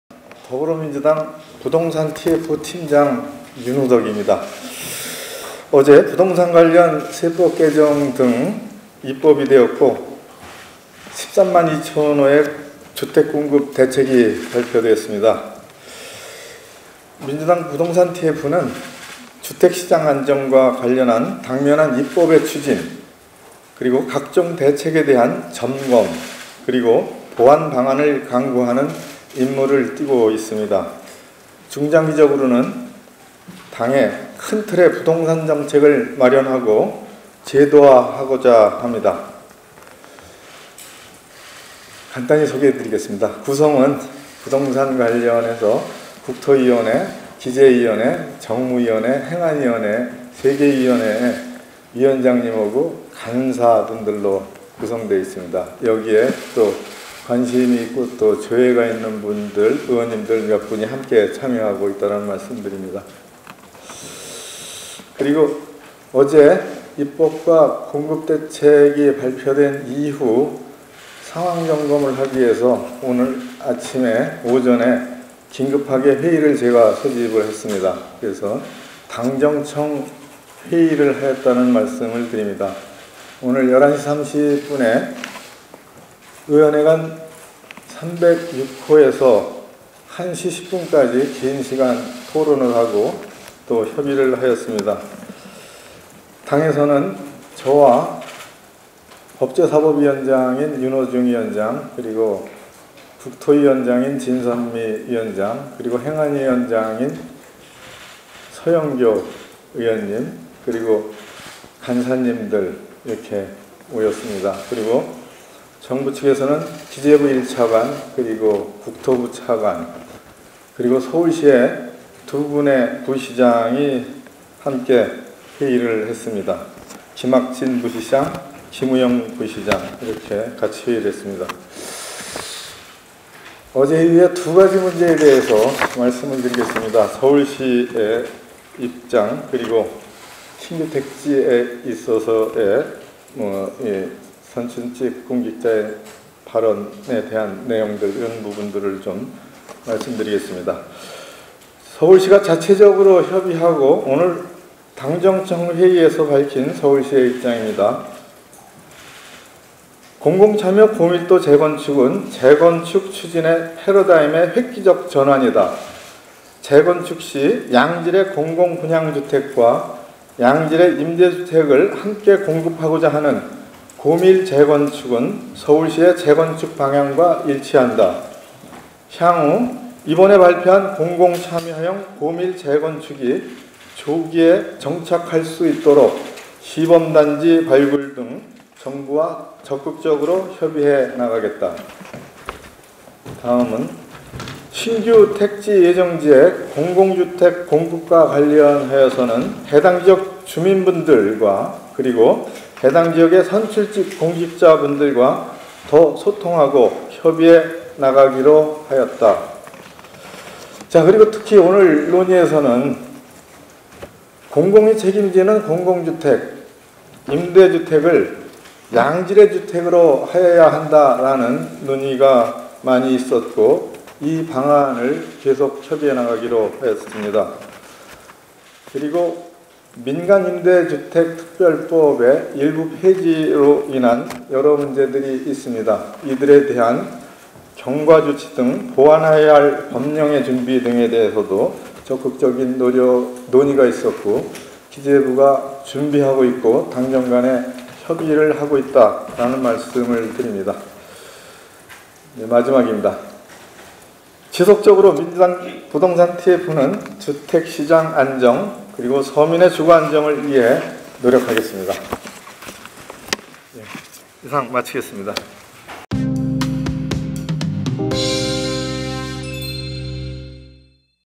‘공급책 갈등’ 봉합에 눈 충혈된 윤후덕 "서울시도 고밀도 재건축 동의"
정부의 8·4 수도권 주택공급책을 놓고 정부·여당과 서울시 간 잡음이 계속되자 더불어민주당이 진화에 나섰다.

윤후덕 더불어민주당 부동산TF 단장은 5일 서울 여의도 국회 소통관에서 부동산 대책 당정 협의 내용 브리핑을 열고 "고밀도 재건축이 서울시 재건축 방향과 일치한다"고 전했다.

공공참여형 재건축도입에 부정적 입장을 보였던 서울시가 이날 열린 당정청 긴급회의 후 전향적인 입장변화를 보인 것이다.